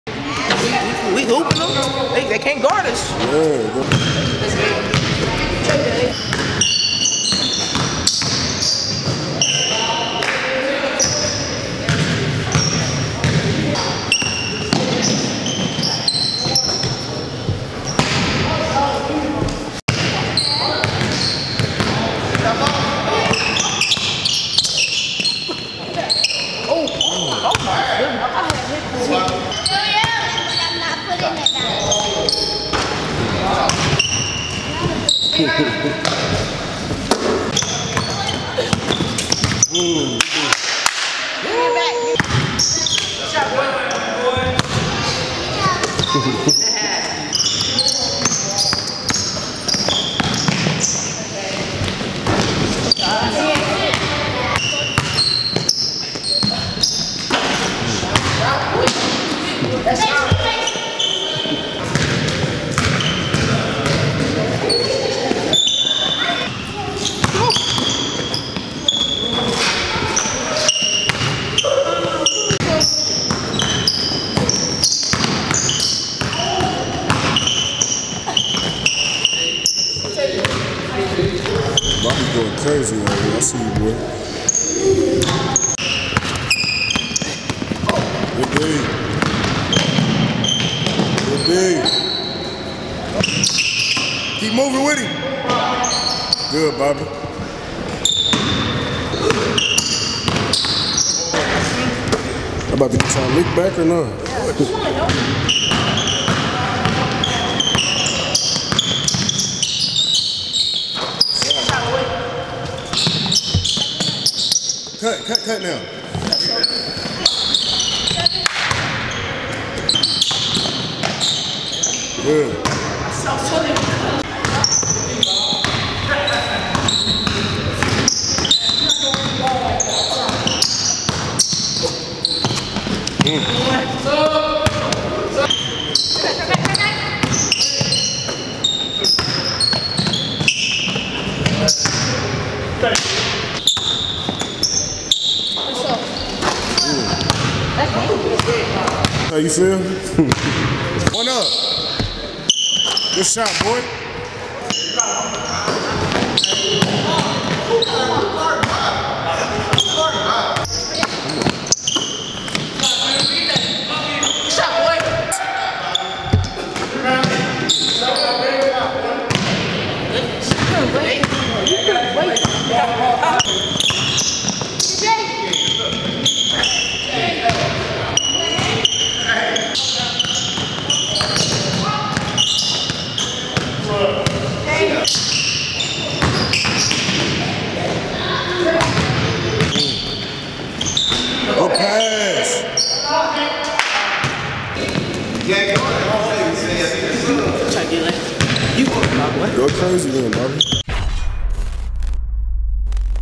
7th grade Bobby Thomas GOES OFF at the Indy Sports Film Open Gym! One of the BEST in the Country!
7th grade Bobby Thomas GOES OFF at the Indy Sports Film Open Gym! One of the BEST in the State!

INDY SPORTS FILM #1 Youth Media Platform in Indiana!

Event coverage and inquires.